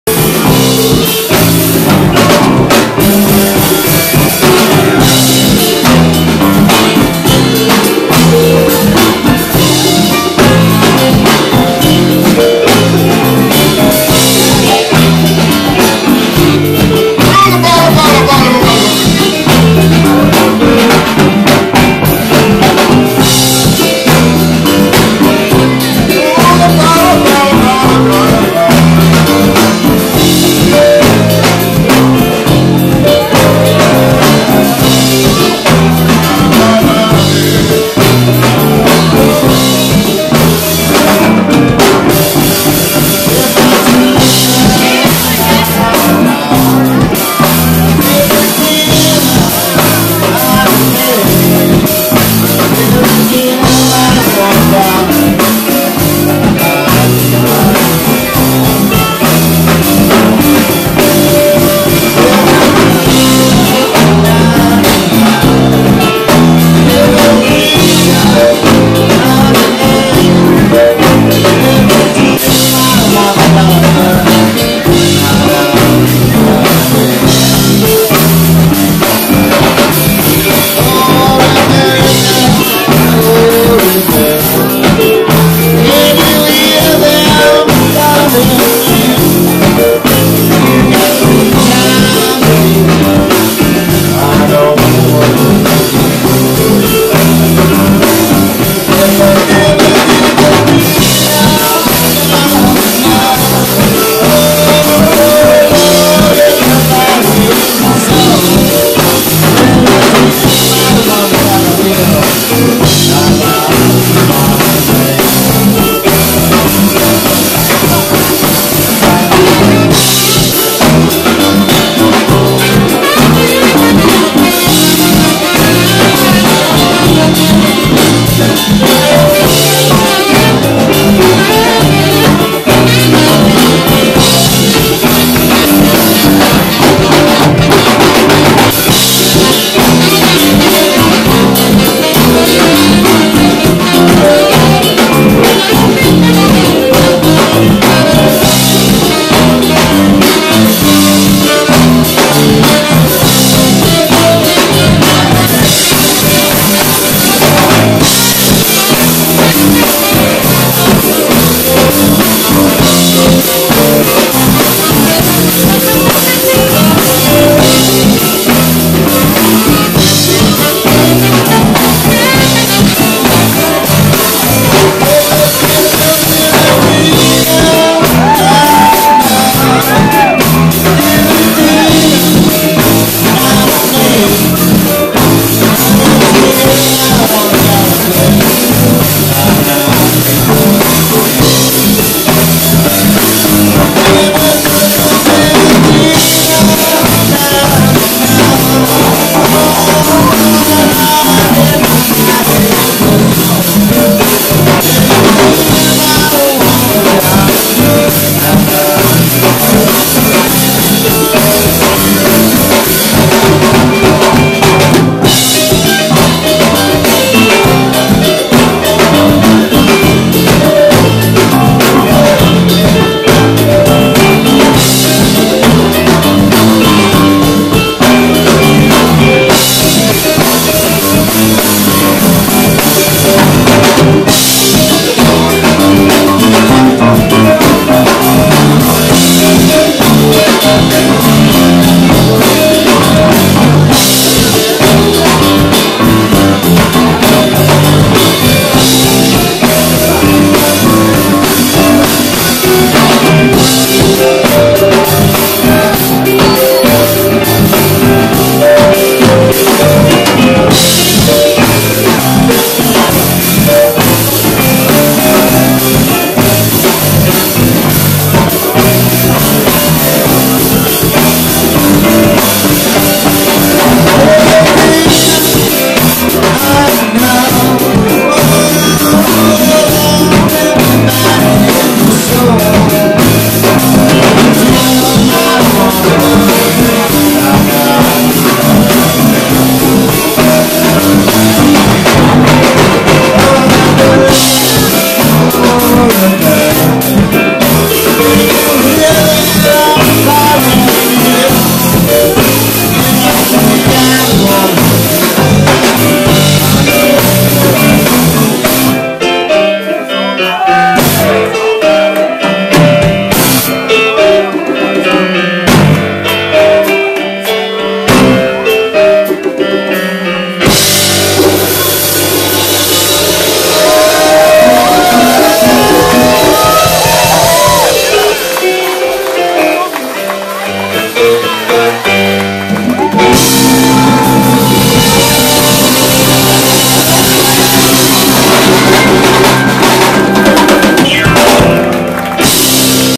JoJo's Mardi Gras Band ~ Zebra Lounge (JC 9)
JoJo and his Mardi Gras Band fills the Zebra Lounge.

An answer provided by this video: there is life after X-Files and the Lone Gunmen, see the Bass Player.

A thing not evident within, JoJo is probably the nicest and most approachable rocker around.  Awesome to see you on the boat and congrats on 25th anniversary of Widespread Panic